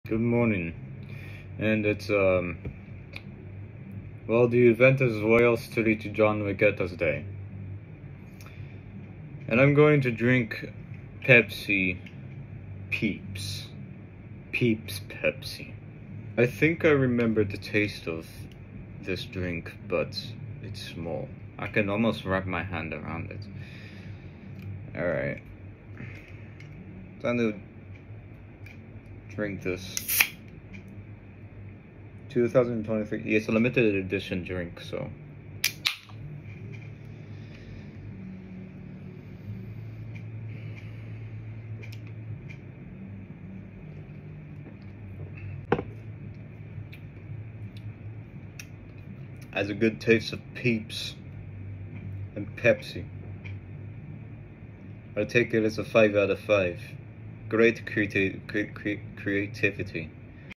Pepsi peeps, peepsi, or pipis.
This is a soft drink that is limited to Canada, I have tasted this before and I like the creativity of what Pepsi and peeps made.

Oh yeah to tell you about the event, The Royal St. John’s Regatta is the oldest organized sporting event in North America. It has been a part of Newfoundland history for over 200 years. The first record of an organized event was in 1816, but rowing matches were common among ships crews in St. John’s Harbour since at least the 1700’s.